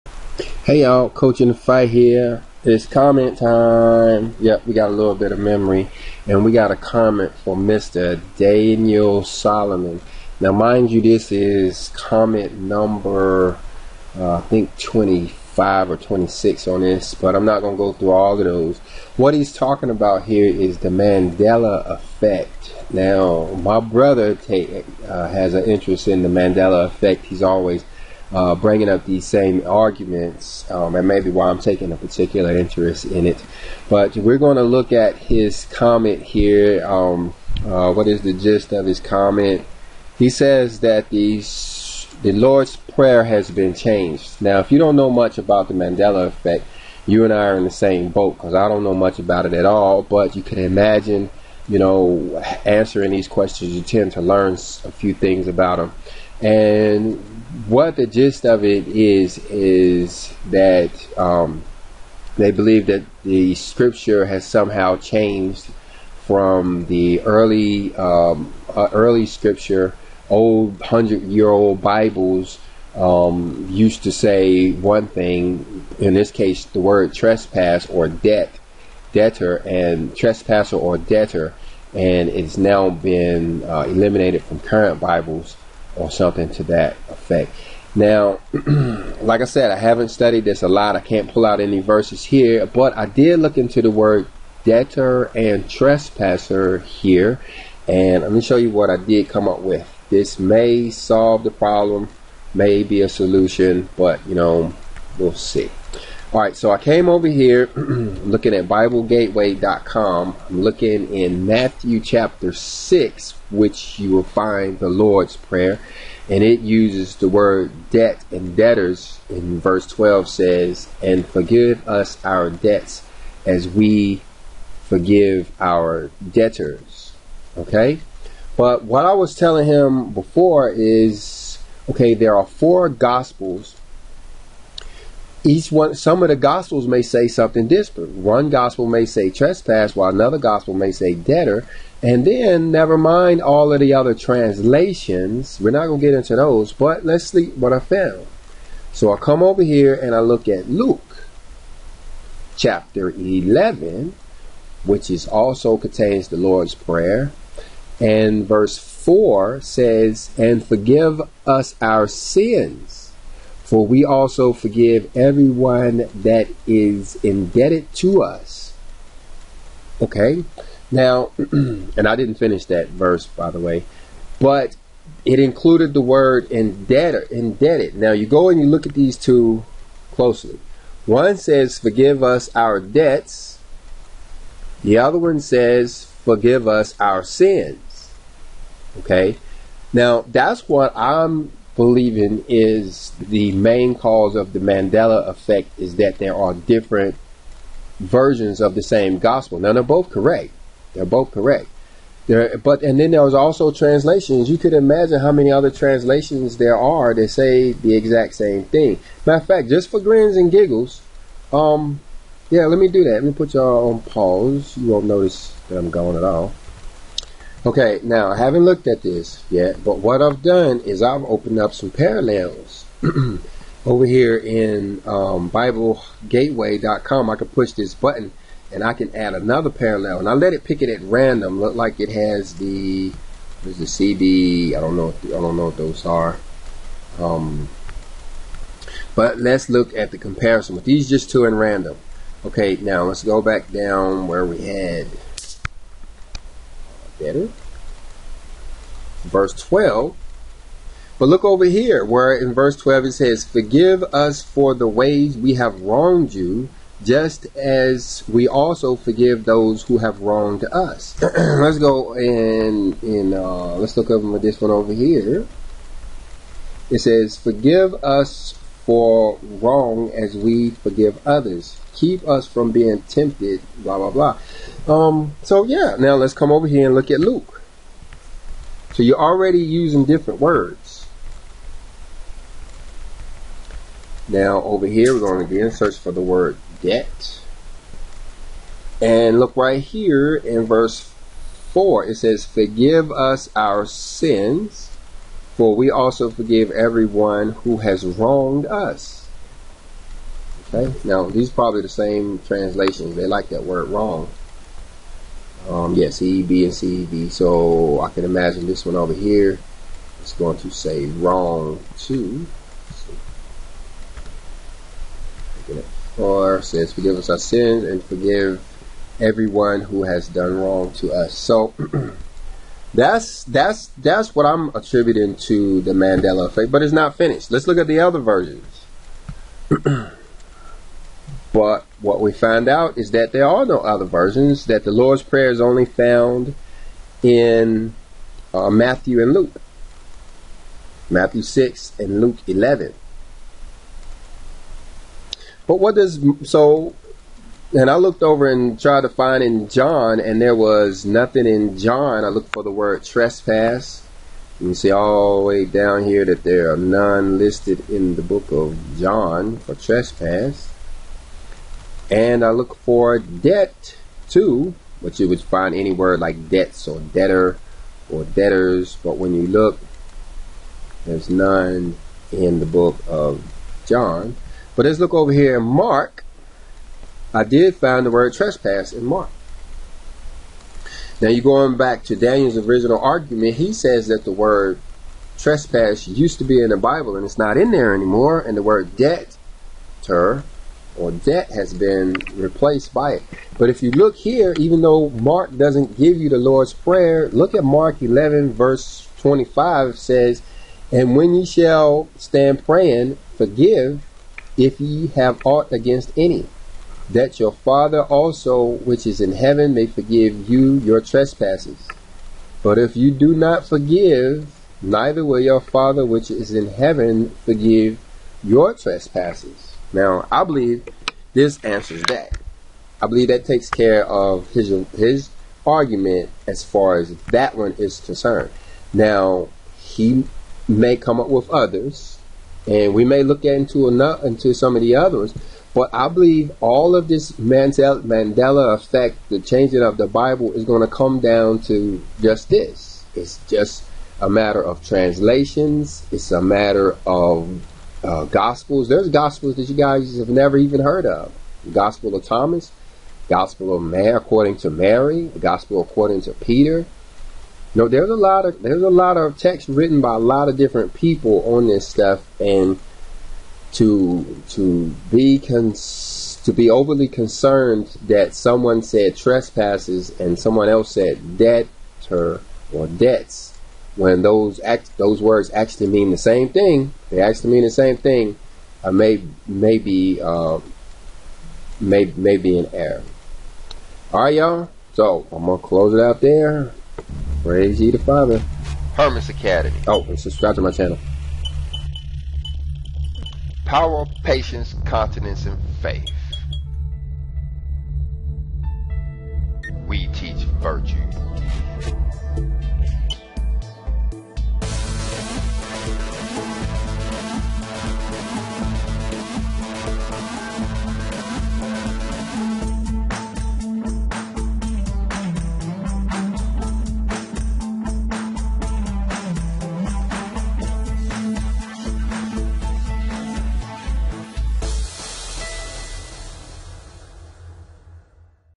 The Mandela Effect on the Bible Explained. How The Bible Was Changed.
A clear explanation of the Mandela Effect as it relates to the Bible.  Yes the Bible was changed.  Here is how.